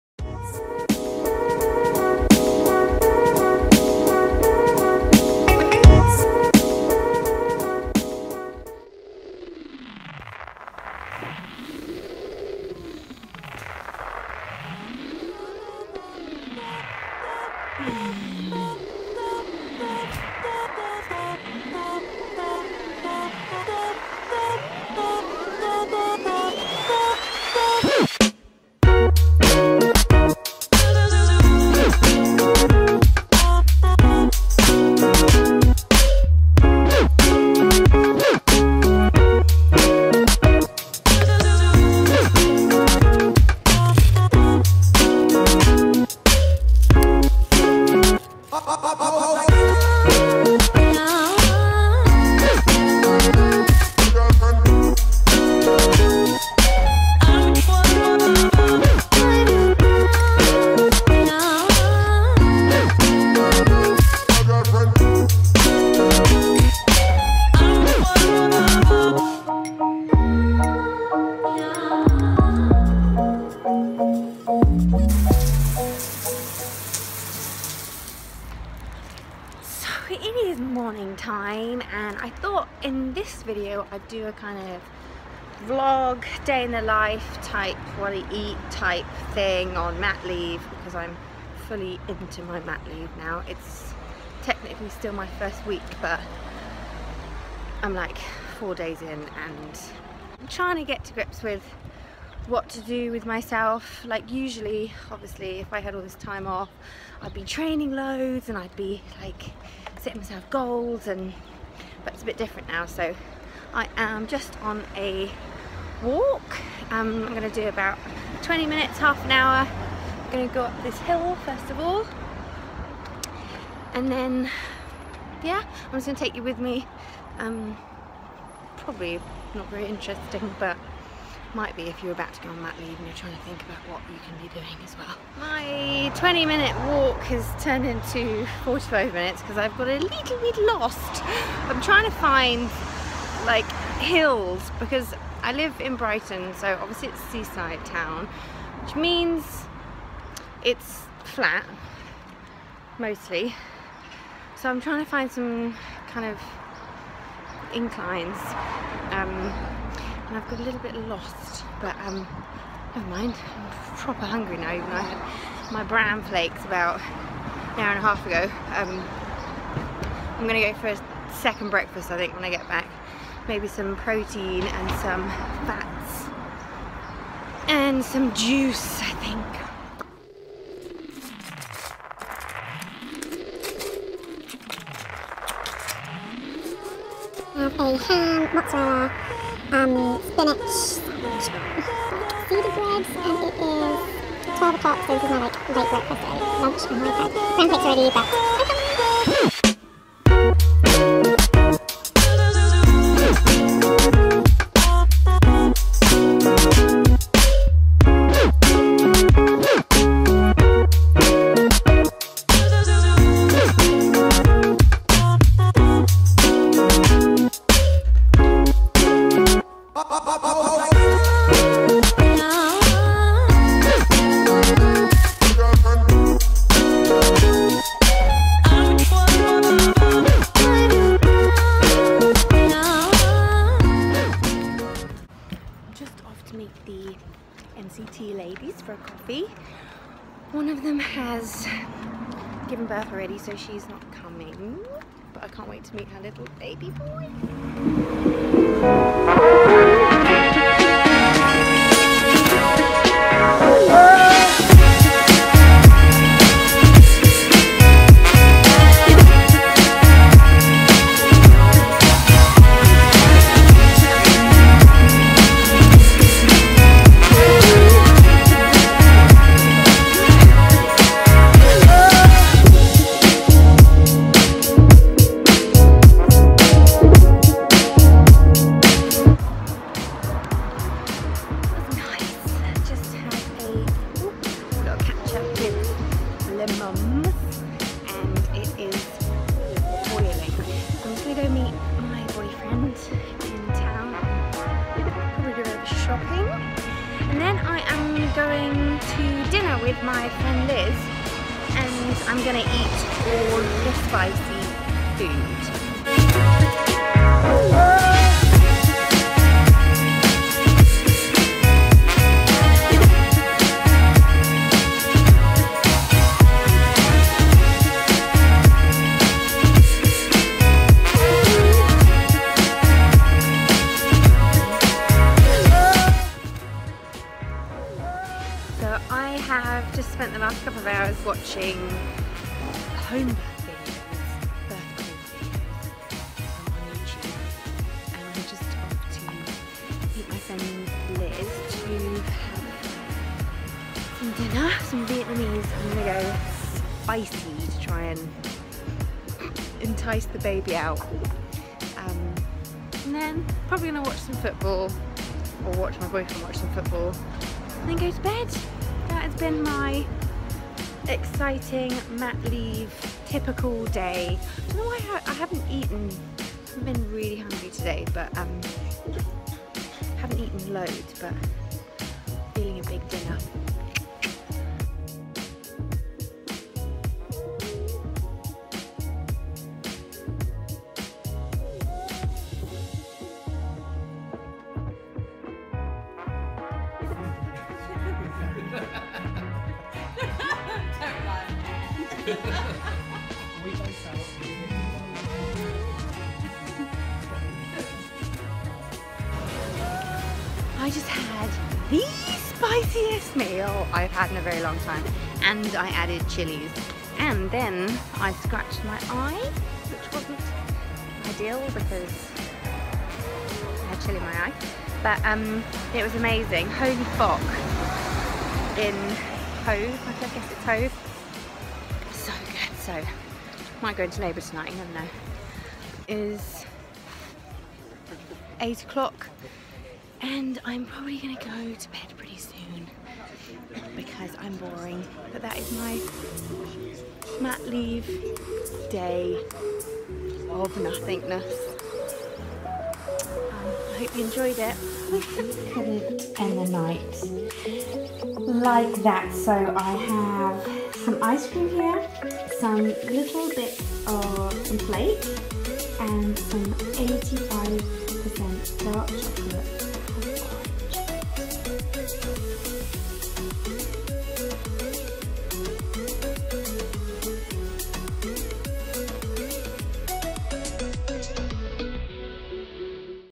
WEEK 39 - WHAT I EAT IN A DAY / MATERNITY LEAVE
I am a week into my maternity leave and Im trying my hardest to relax and make the most of the time off!

This 'day in the life' video shows you a typical day at 39 weeks.
What I eat and what Im up to :)